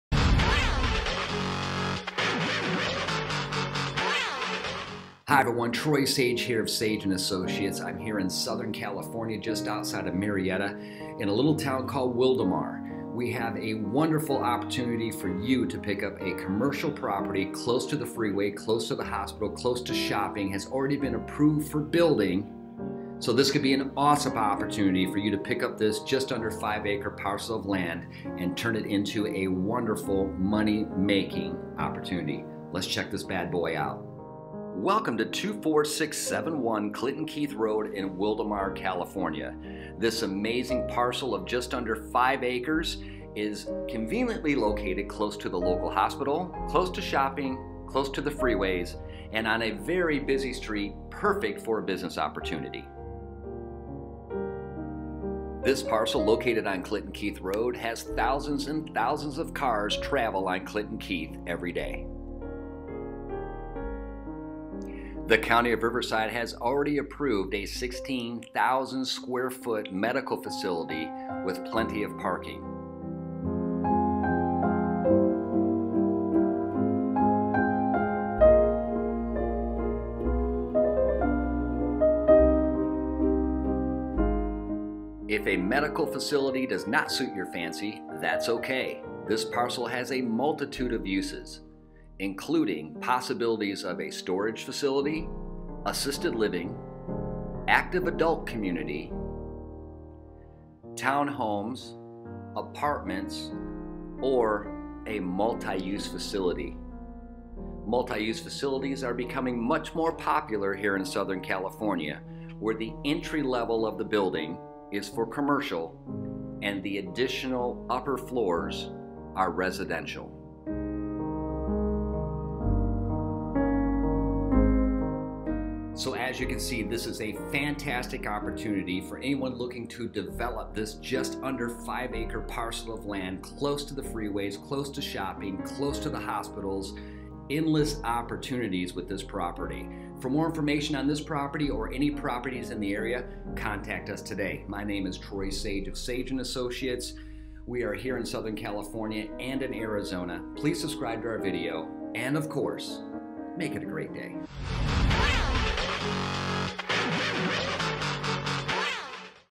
COMMERCIAL LAND - 24671 Clinton Keith Road
PRICE REDUCED: $850,000

Wonderful Opportunity to Purchase Commercial Property! Just Under 5 Acres!
- Approved to Build and Develop
- Close to Freeways
- Close to Shopping
- Endless possibilities

Troy BRE: 01512201